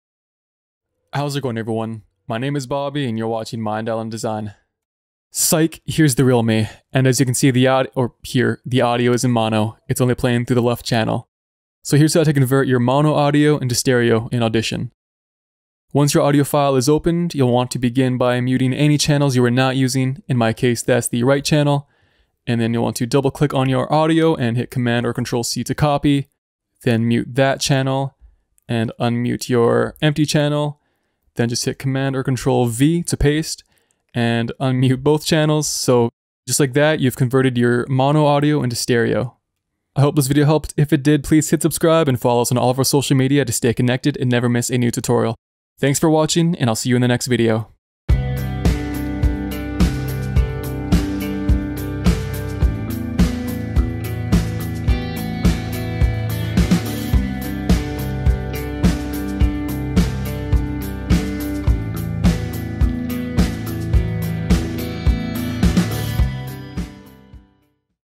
Convert Mono Audio to Stereo in Audition
A quick and easy way to convert mono audio to stereo in Adobe Audition.